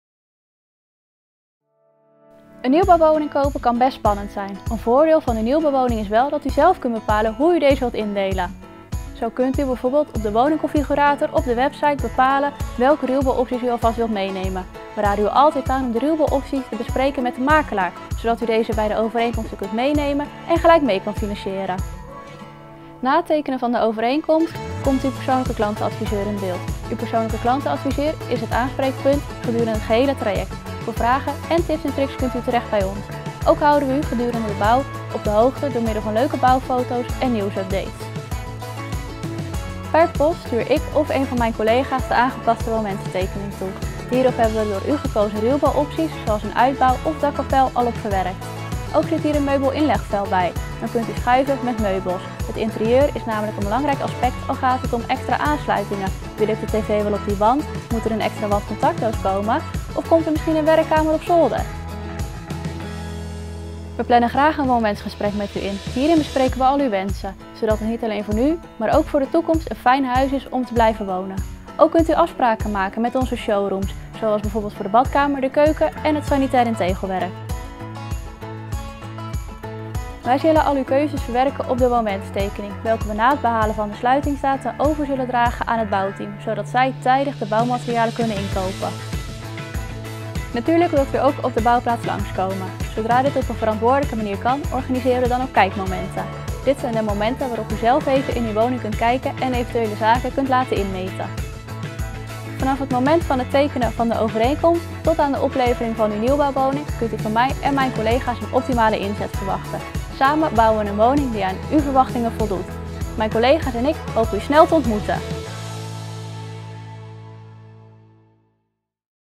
Klantadvies bij Gebroeders Blokland | Ons Dorp
Een nieuwbouwwoning kopen kan best spannend zijn en er moeten veel keuzes gemaakt worden. Onze klantadviseurs staan voor u klaar om u te begeleiden bij het maken van die keuzes. In deze video vertellen we wat u te wachten staat.